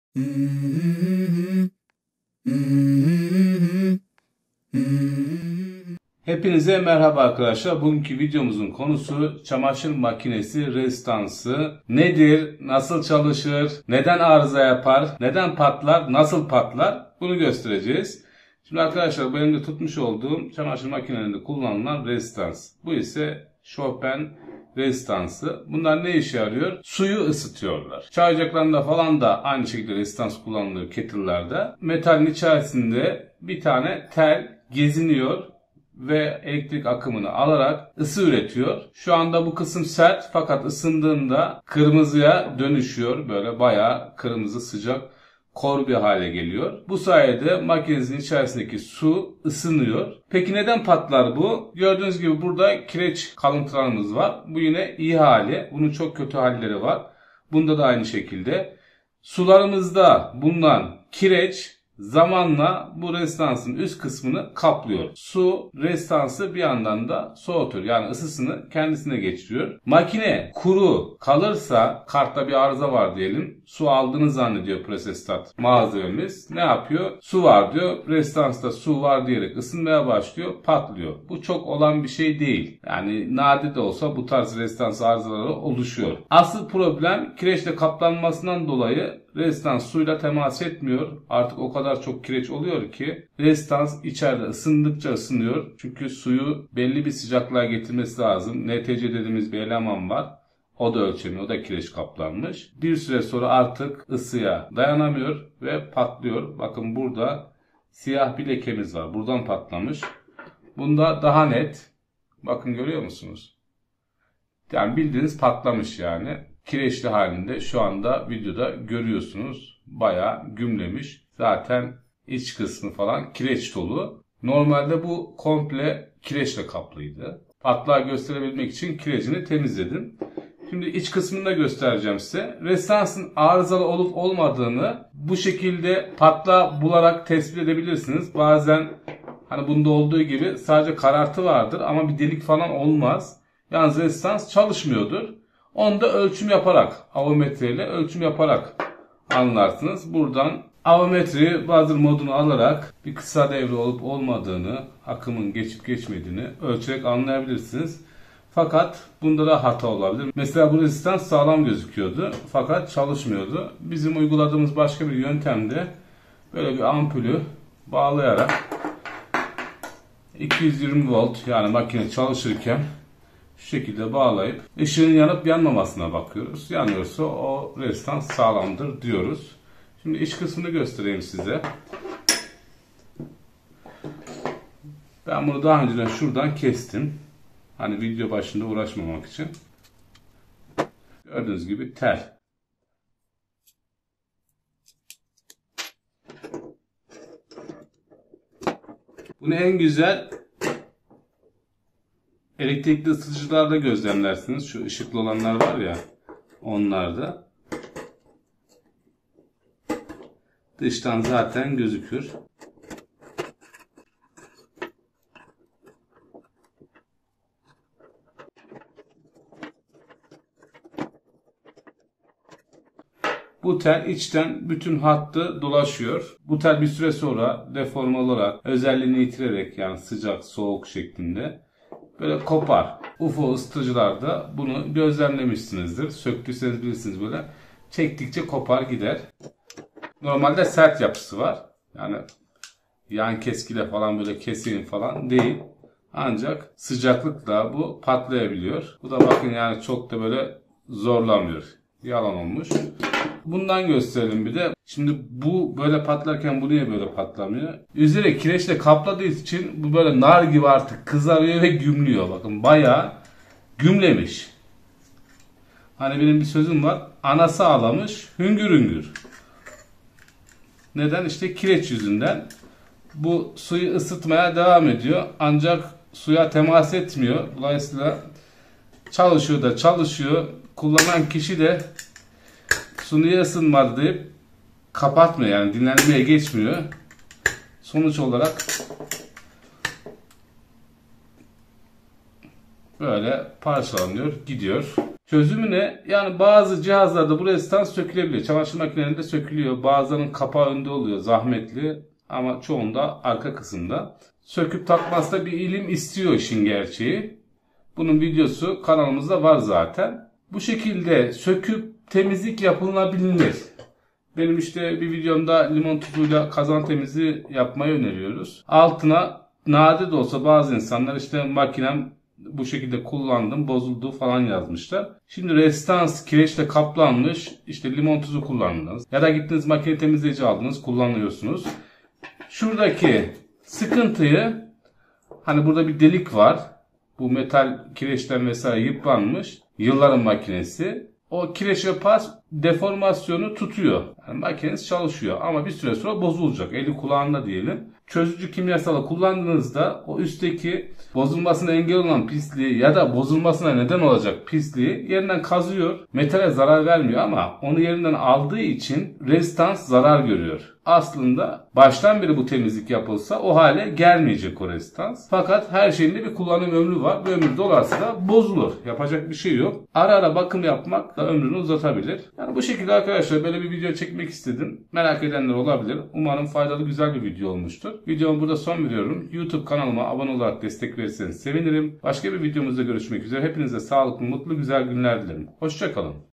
Rezistansların İç Yapısı Nasıldır? Neden Arızalanır? Tamiri Edilir mi?
Çamaşır makinelerinde de kullanılan rezistanslar neden arızalanır? Arızalanmaması için ne yapmalıyız ve rezistansları nasıl kullanmalıyız? Şofben/termosifon rezistansları ve çamaşır makinesi rezistansının iç yapısı nasıldır? rezistanslar nasıl çalışır? Cevapları videomda.